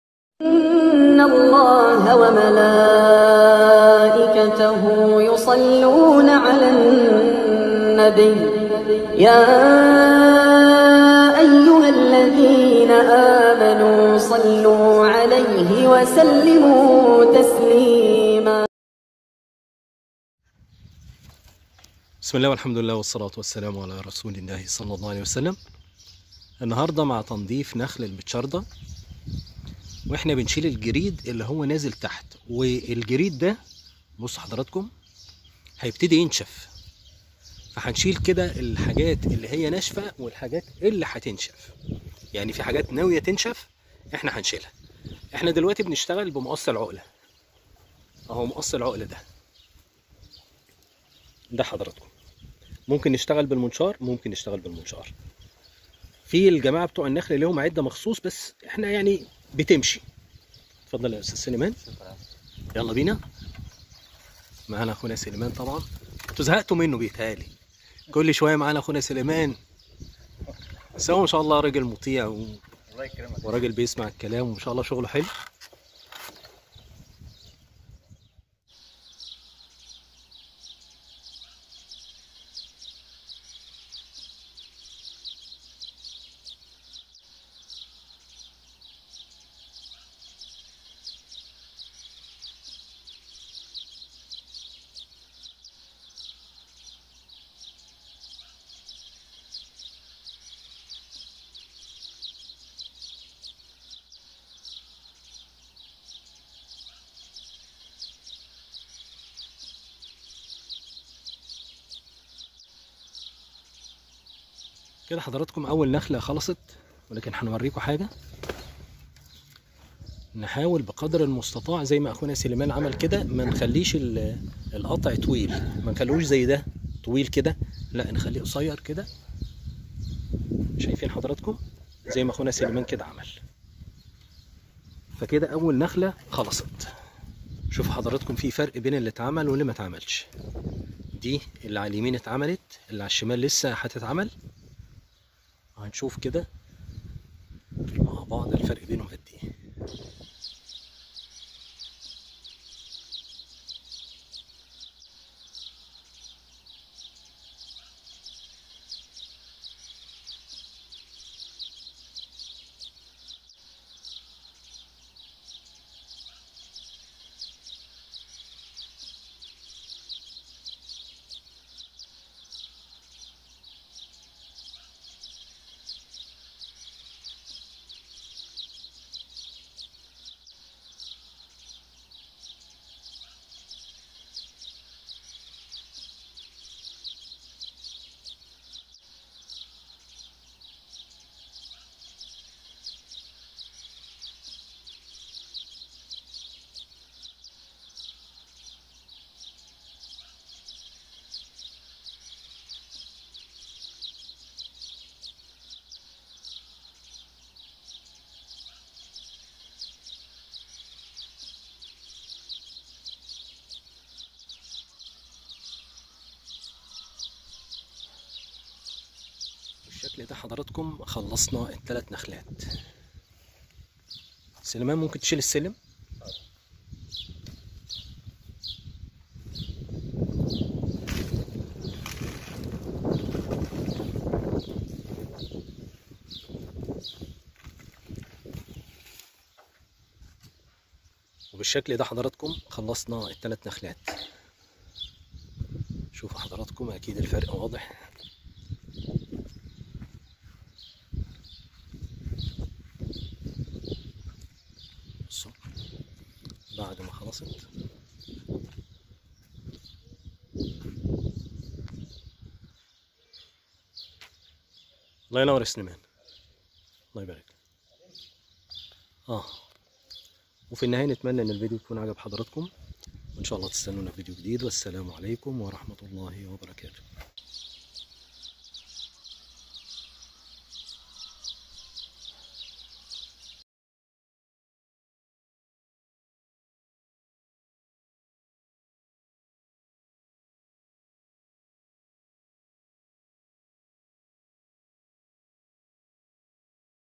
طريقة تقليم نخل بتشردا من الأوراق الصفراء Palm pruning method. of yellow leaves
يوم من شغلي تقليم  نخل بتشردا. من الجريد الناشف و الأوراق الصفراء
 وذلك باستخدام مقص العقله او المنشار وكمان نحتاج سلم
 طريقه التقليم
نقوم بتقليم الاوراق الجافة و  الصفراء
 وكمان نقلم الجريد الناشف. و ضروري عند التقليم نقطع ونترك جزء صغير من الجريد حتي نجعل الشكل يكون جميل

Arrange _ your_garden_with_Mohamed_Kamal One day of my work, pruning palm trees. Of dry leaves and yellow leaves Using cutting scissors or a saw, we also need a ladder pruning method Types of dry leaves and yellow leaves We also pen dry newspaper. It is necessary when pruning to cut off a small part of the leaves in order to make the shape beautiful

فيديوهات مهمة للعناية بالحديقه